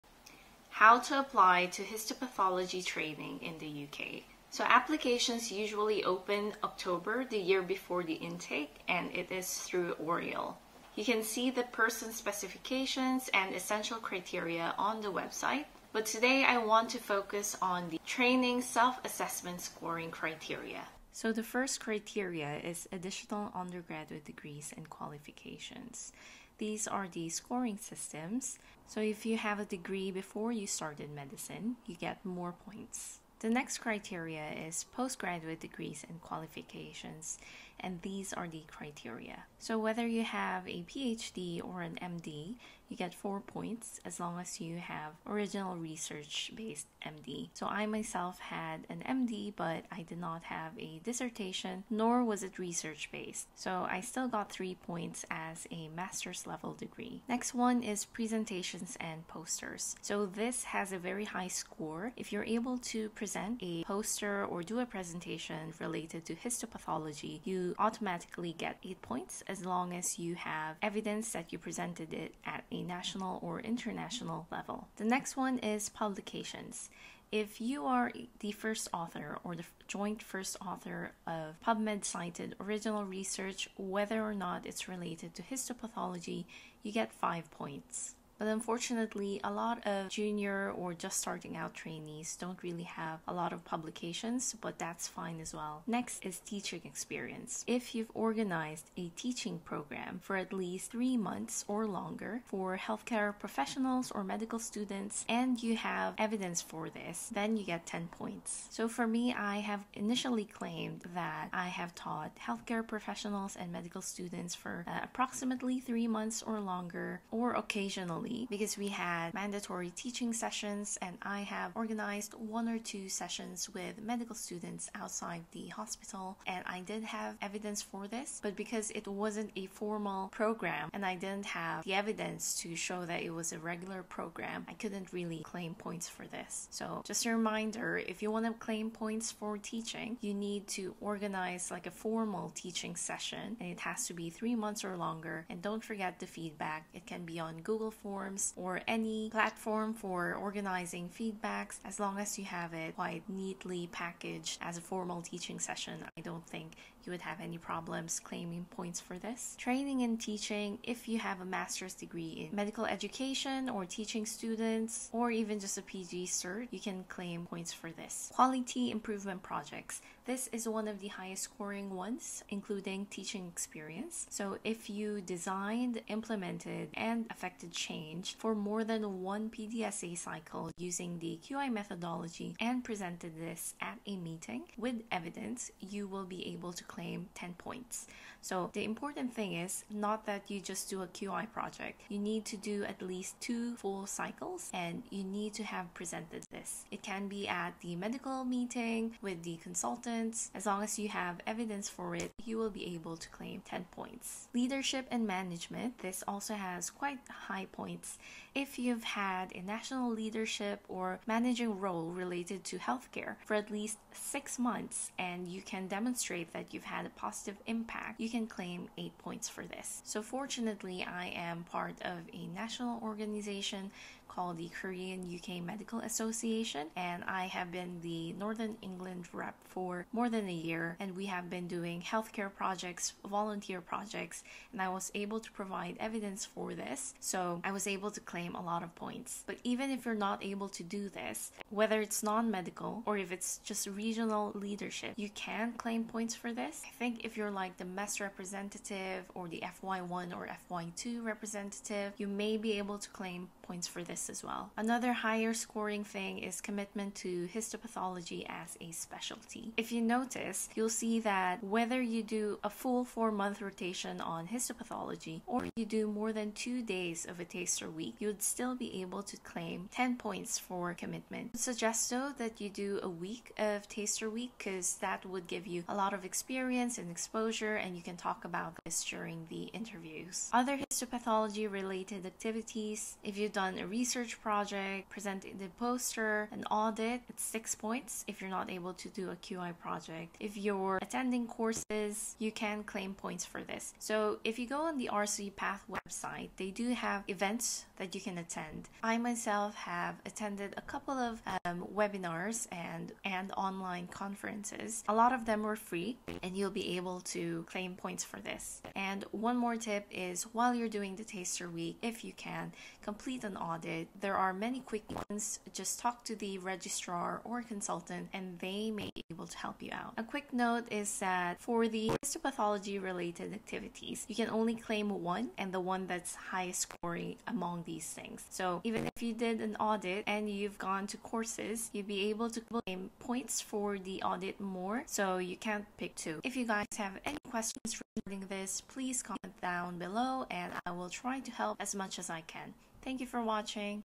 How to apply to Histopathology in the UK (self assessment scoring guide)
In this video, I go into detail on how to get a high score in the self assessment part of the UK Histopathology specialty training applications.

All information is based on the 2024 Histopathology Training Self-Assessment scoring guidance for applicants from the NHS England website